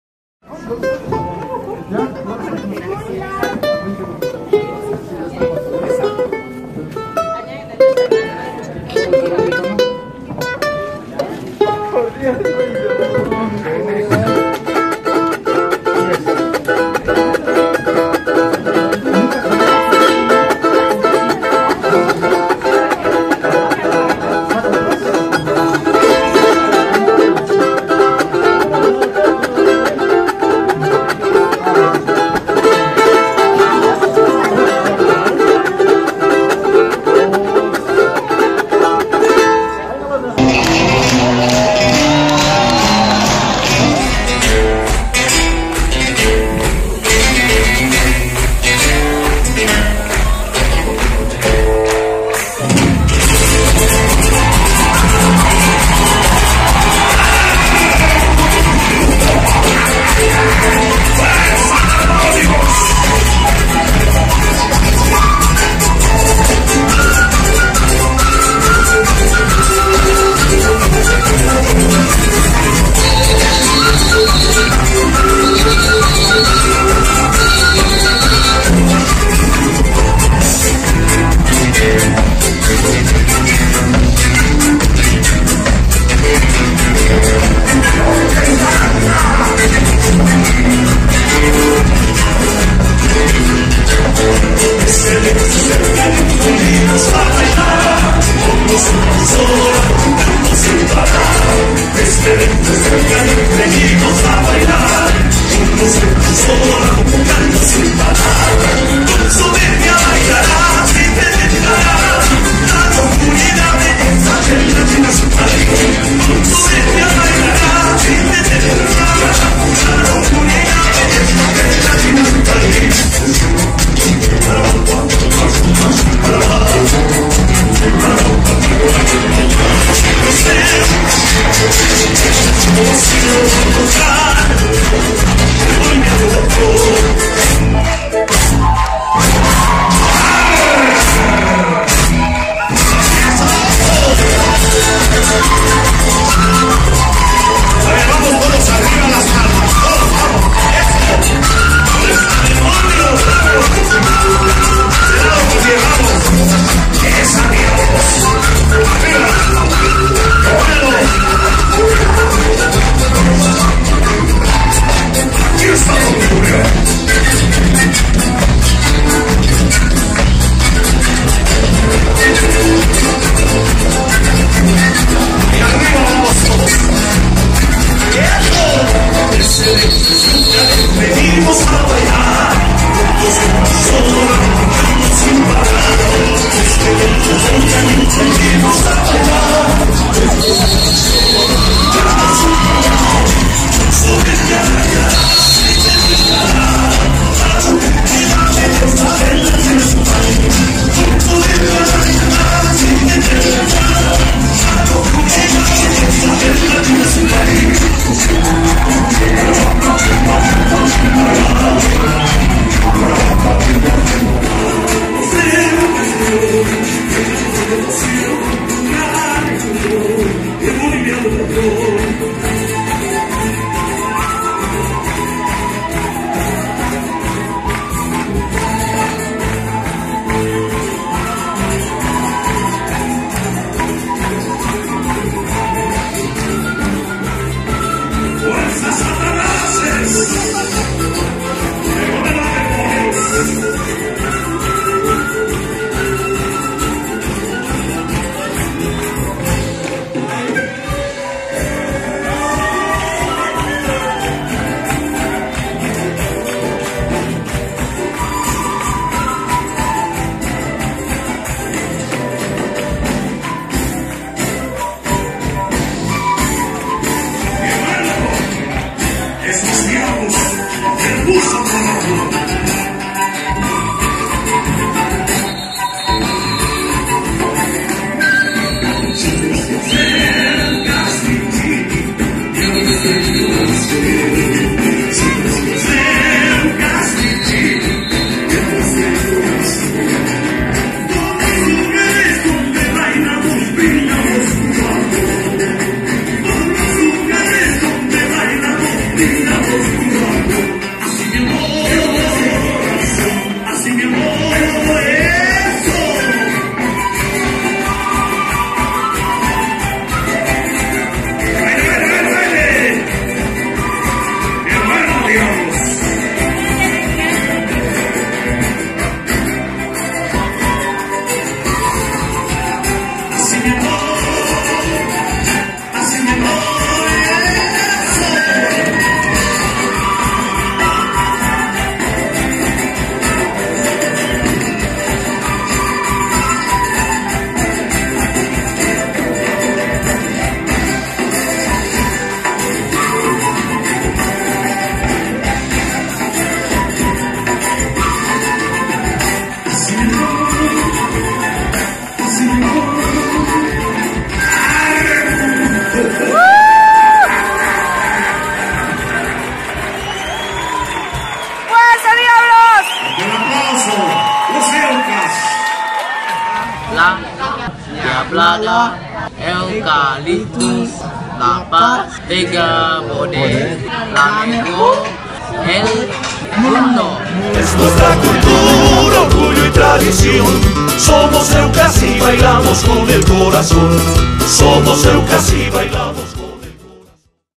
Llajtaymanta y Los Eucas La Paz - “Rumbo a los 30 años” 2015
Concierto de Llajtaymanta “Rumbo a los 30 años” realizado en La Paz, Bolivia, con la participación de la Fraternidad Diablada Eucaliptus La Paz del G.P.
Teatro Municipal Alberto Saavedra Perez, La Paz Bolivia